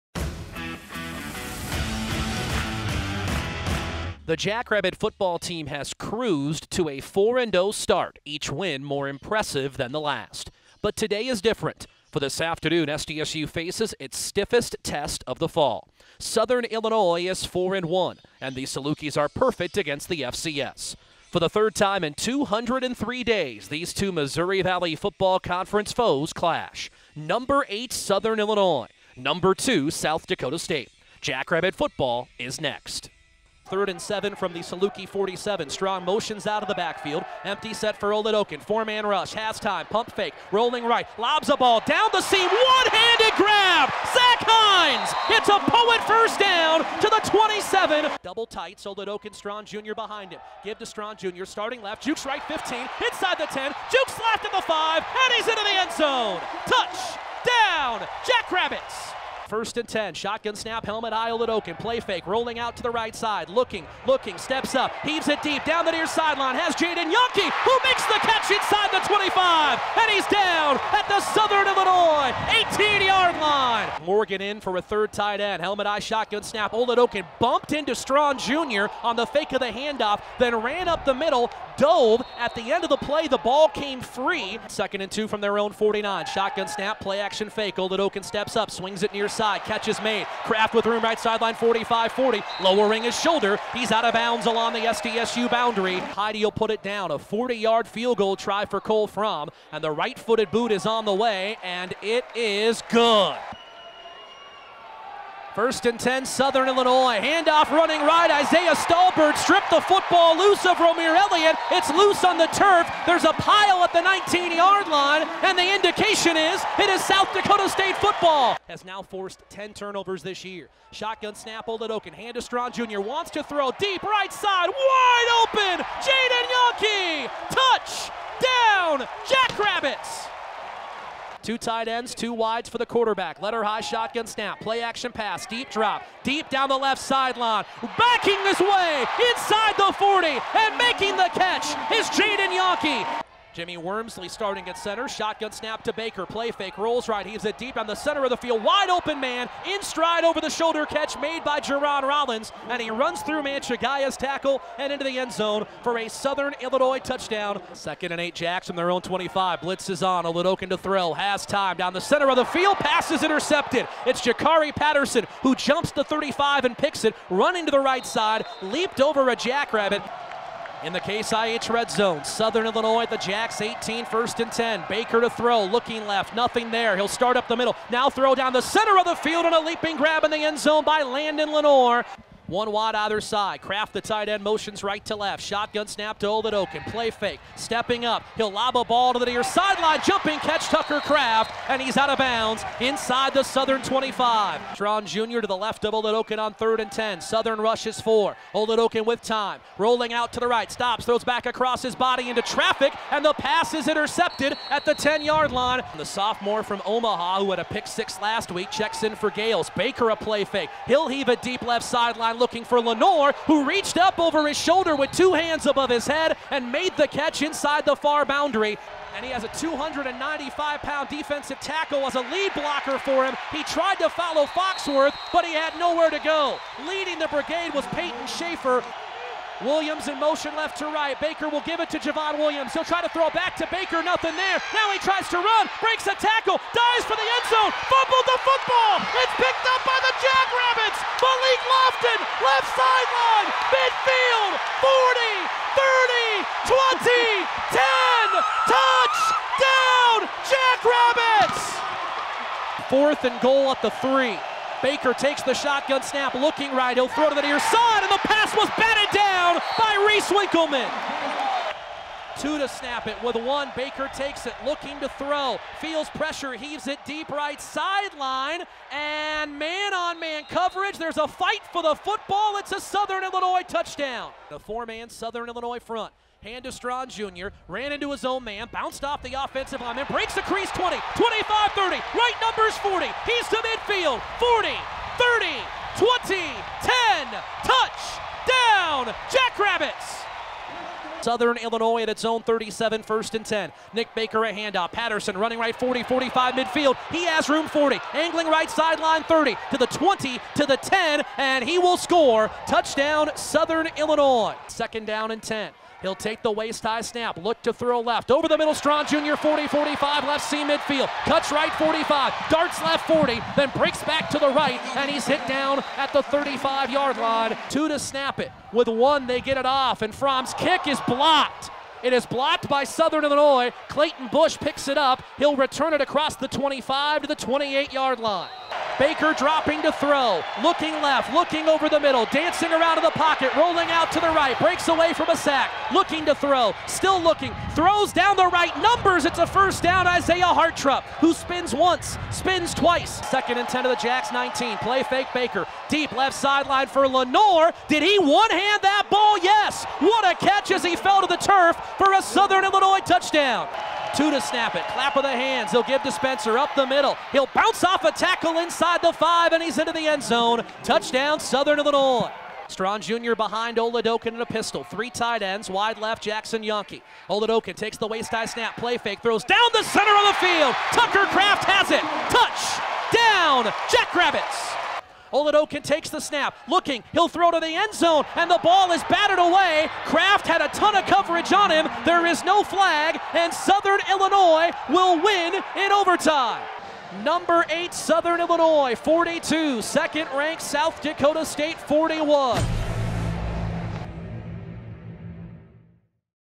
Football Highlights vs Southern Illinois (10.09.2021)
The Jacks lose a heartbreaker in overtime to Southern Illinois, 42-41.